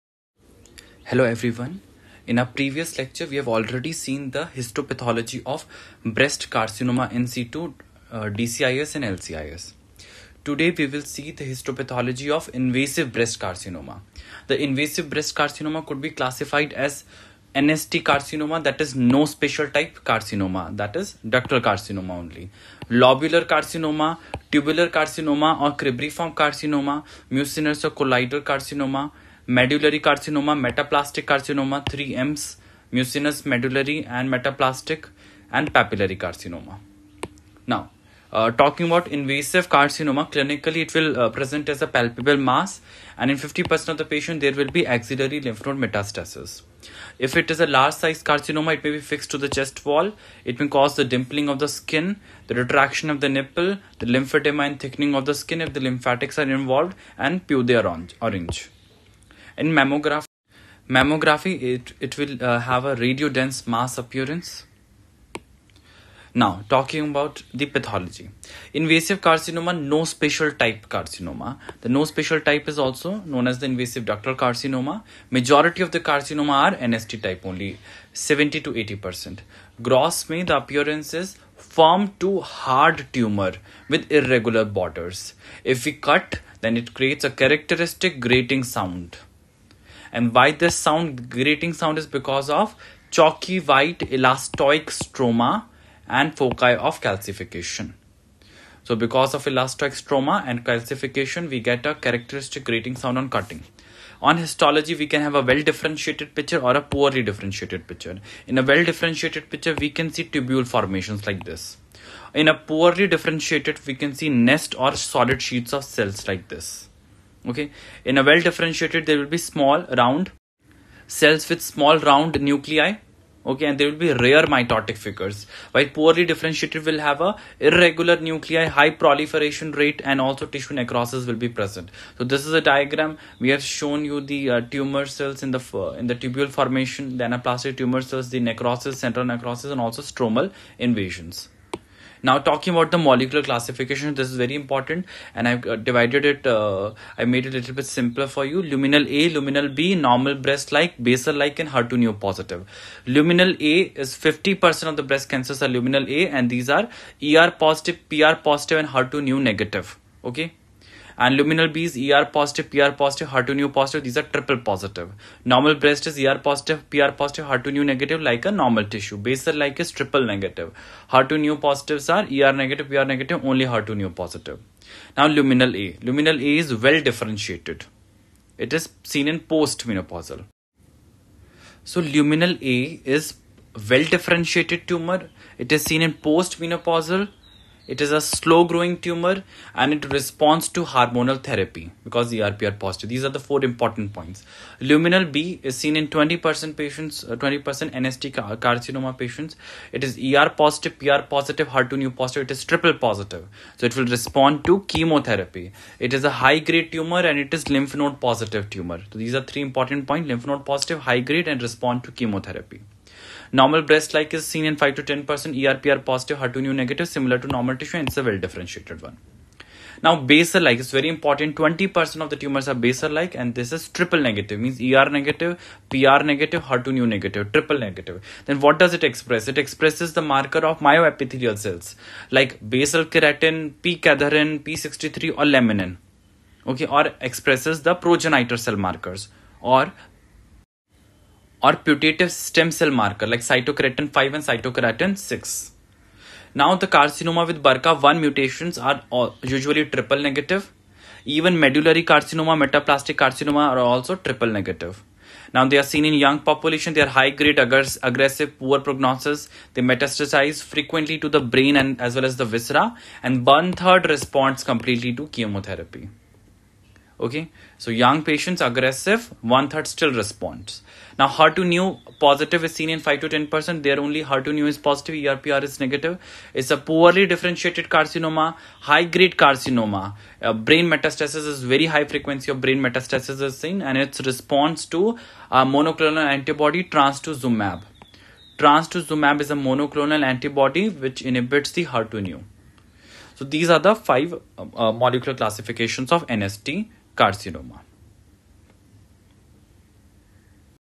Histopathology of Invasive Breast Carcinoma- part 1
Pathology Lectures
Invasive Breast Carcinoma
Histopathology
NST No special Type - Molecular classification
Lobular carcinoma
Medullary Carcinoma
Colloid or mucinous Carcinoma
Tubular or cribriform carcinoma
Papillary Carcinoma
Metaplatic carcinoma